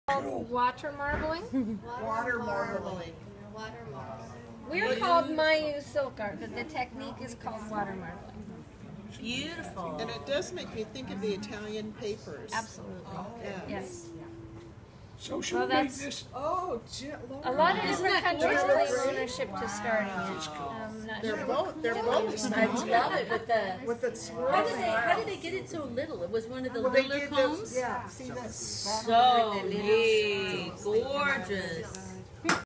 We always do a demonstration before the party starts, so everyone can see what they'll be doing. Of course, every design is different. Watch this with the sound turned on to hear the comments!